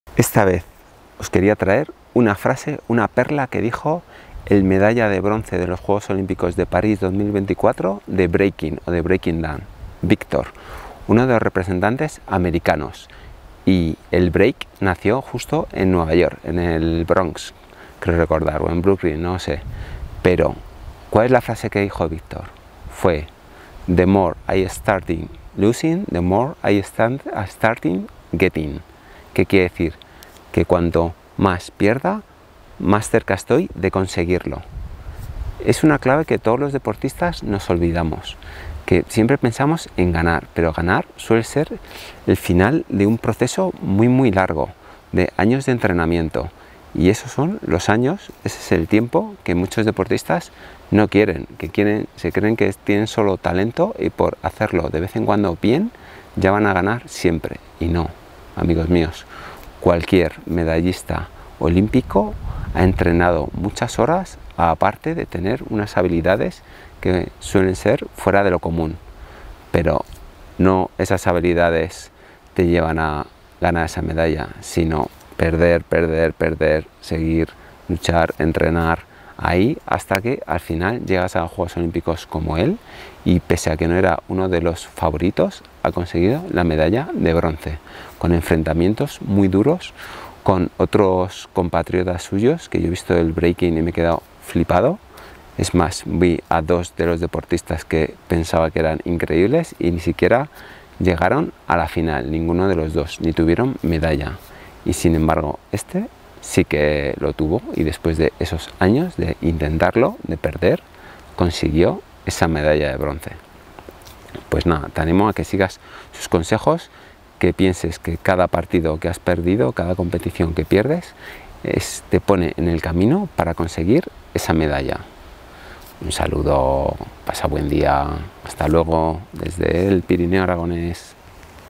Clave deportista olímpico Victor Breaking JJOO Paris
El camino hacia la medalla de bronce de Victor en el Breaking de los JJOO Paris 2024.